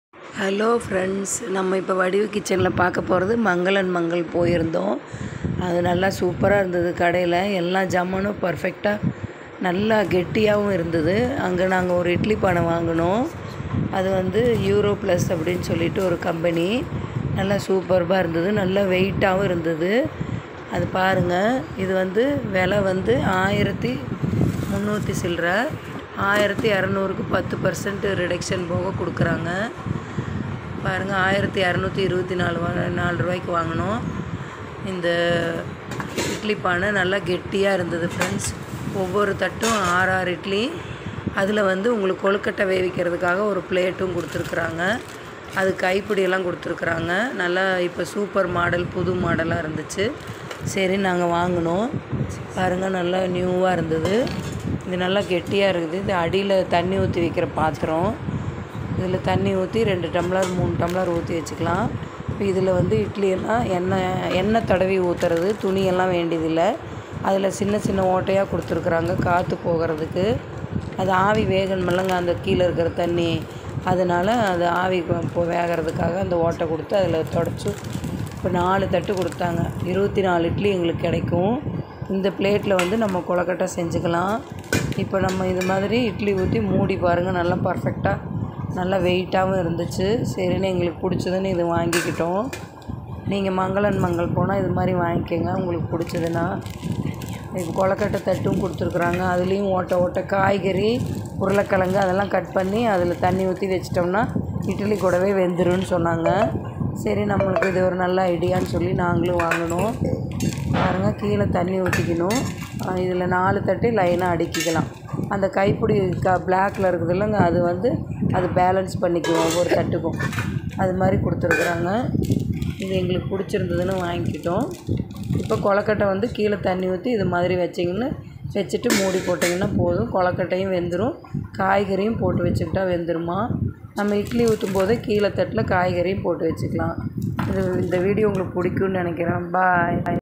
vadivu kitchen#shorts //வாங்க மங்கள் அண்ட் மங்கள் பார்க்கலாம்
இட்லி பாத்திரம் வாங்க போனோம் அந்த இட்லி பாத்திரம் எப்படி இருக்கு பார்க்கலாம் வாங்க வீடியோ குள்ள போய் பார்ப்போம்